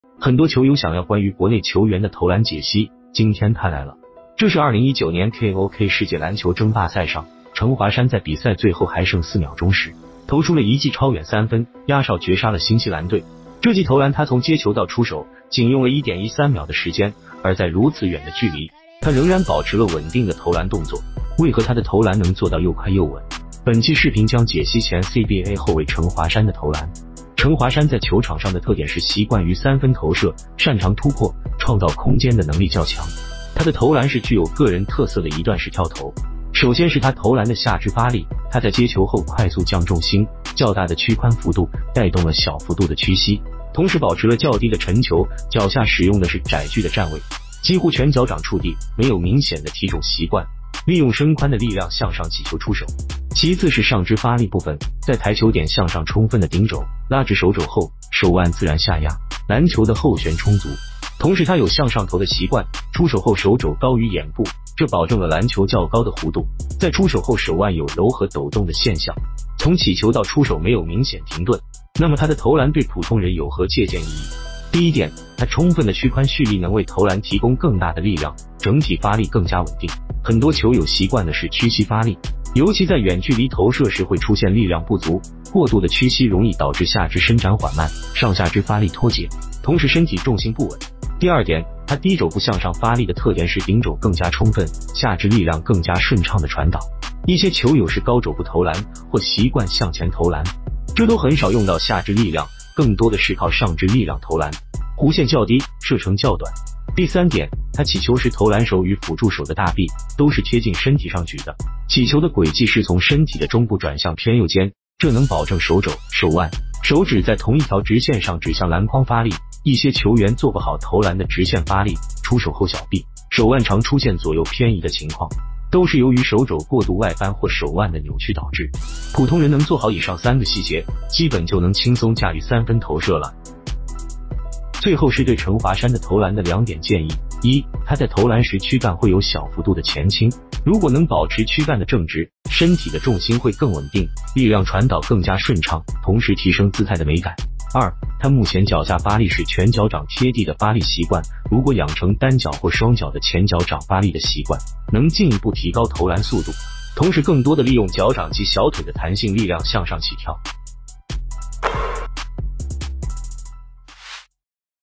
干货纯享！适合普通人的投篮模型，前CBA后卫程华山的投篮解析
本频道制作的所有视频都是体育赛事的精彩瞬间，与暴力无关。 很多球友想要关于国内球员的投篮解析，今天它来了！ 这是2019年 KOK 世界篮球争霸赛上，程华山在比赛最后还剩四秒钟时，投出了一记超远三分，压哨绝杀了新西兰队。这计投篮他从接球到出手仅用了1.13秒的时间，而在如此远的距离，他仍然保持了稳定的投篮动作。为何他的投篮能做到又快又稳？本期视频将解析前CBA后卫程华山的投篮。